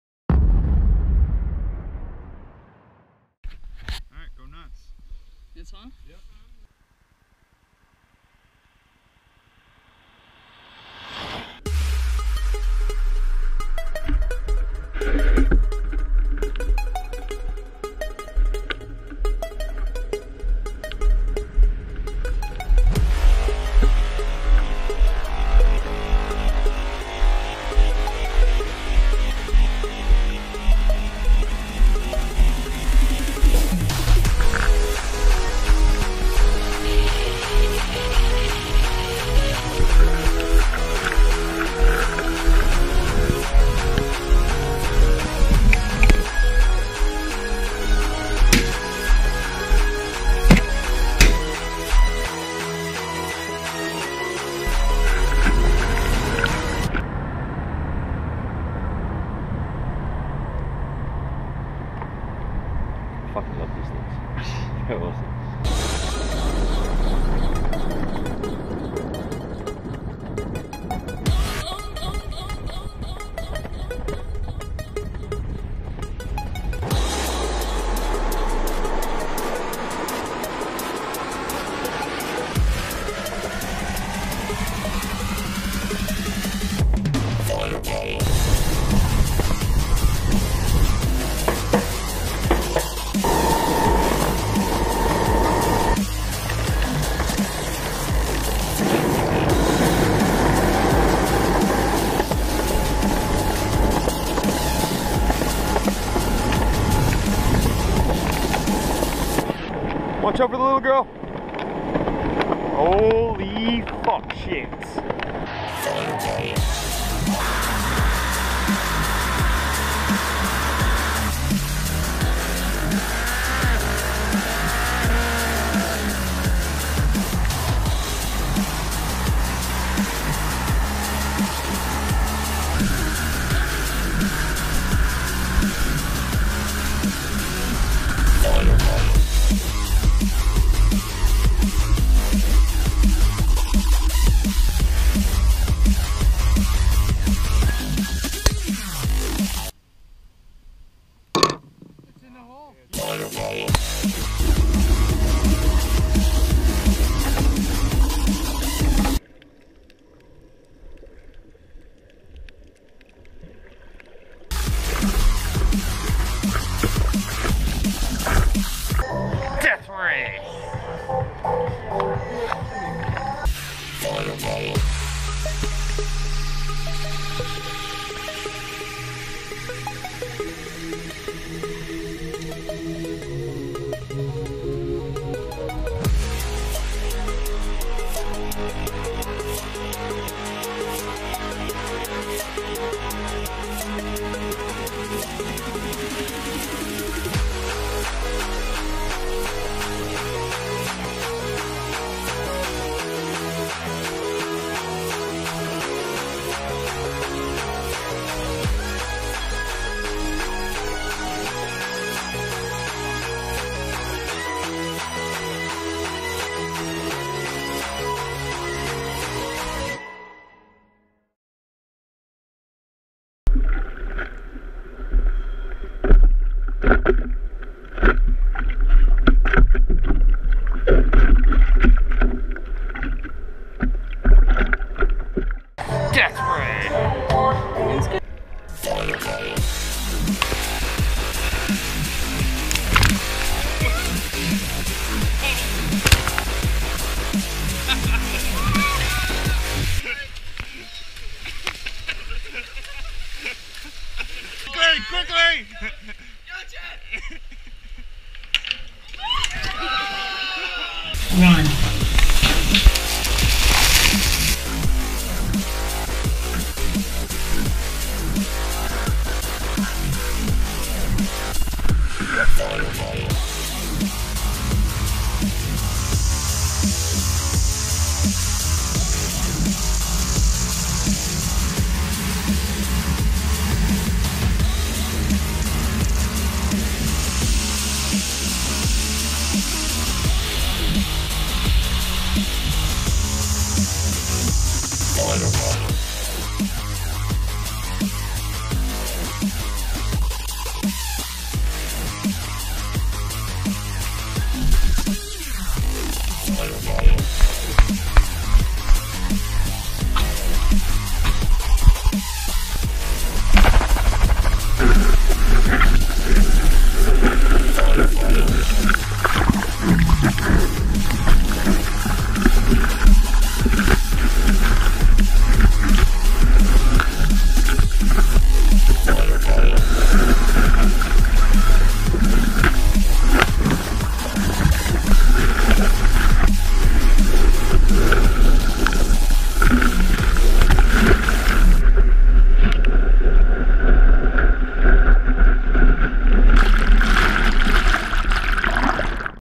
The GoPro Experience 2
A compilation of some of my footage - using the GoPro Hero3&4 Black Edition.

Footage:
Ontario, Alberta, BC - Canada.
Queensland - Austrailia
Varadero - Cuba